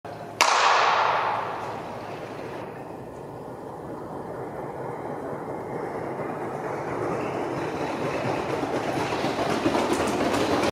Mohai, Rapai, Nádházy és Molnár futása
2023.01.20. Budapest, Lantos Mihály Sportcentrum, 4x400 m-es válogatott váltók 3x3x80m felmérője